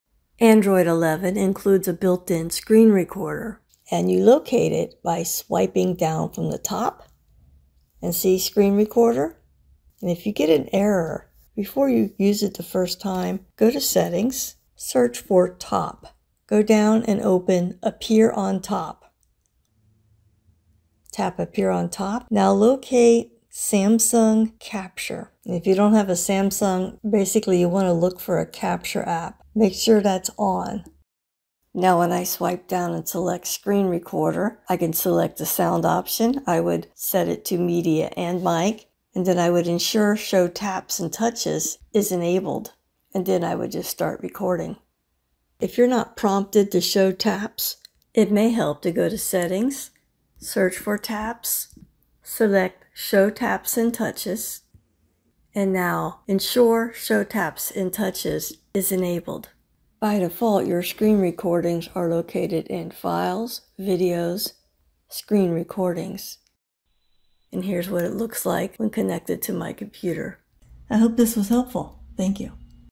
Built-In Screen Recorder in Android OS - Enable and Use
Android 11 and above includes a building in screen recorder / capture.  Phone used is Samsung, but others should be similar.
Show taps and touches.  Enable Samsung Capture to appear on top.
 Allow capture.  error message